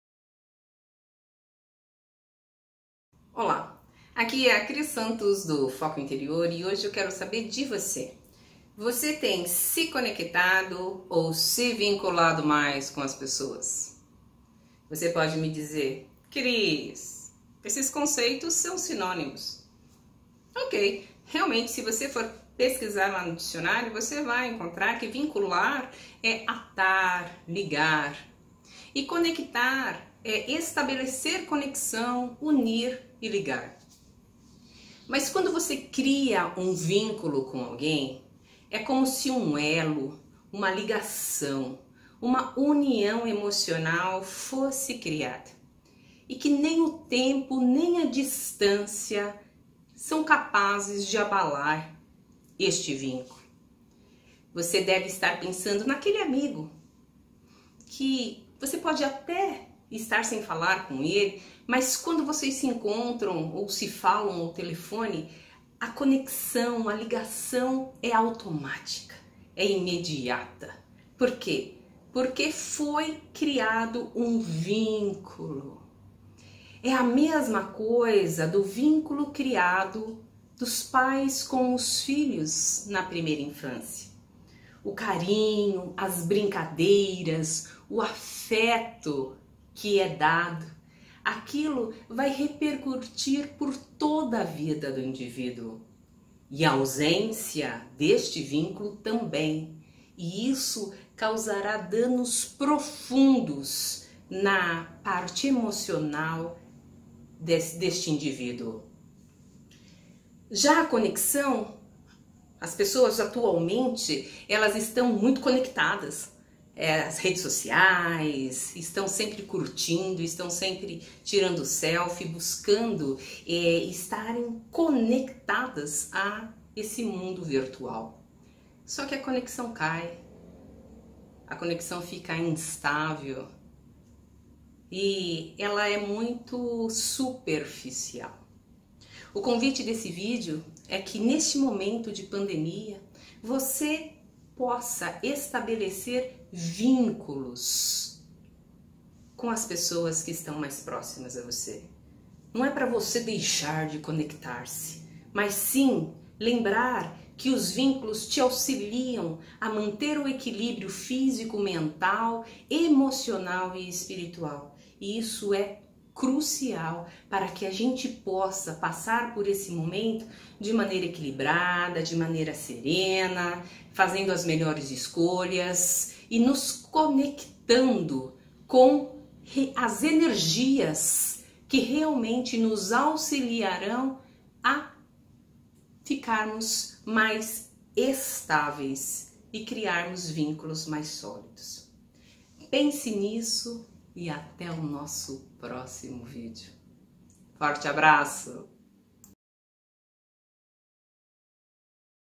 150| Vínculos - Foco Interior
Olá! Aqui é a Cris Santos do Foco Interior e gostaria de saber se atualmente você tem se conectado ou se vinculado mais?